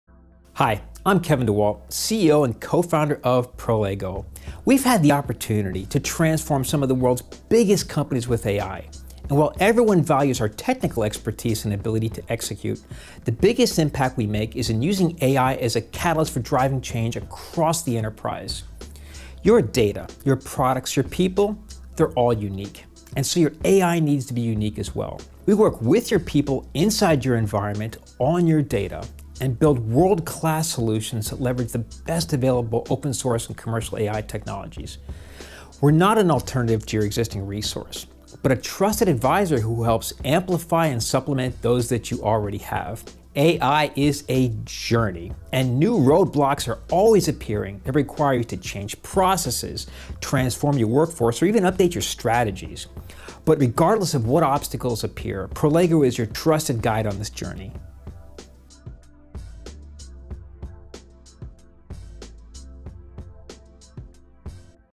Prolego - Trusted Advisors for your AI Journey
We are an elite team of AI engineers and creative technologists committed to guiding the world’s largest companies through their AI transformation.

We do this by building custom, innovative solutions to solve complex business problems. We also help drive the cultural and process changes necessary to fully leverage AI.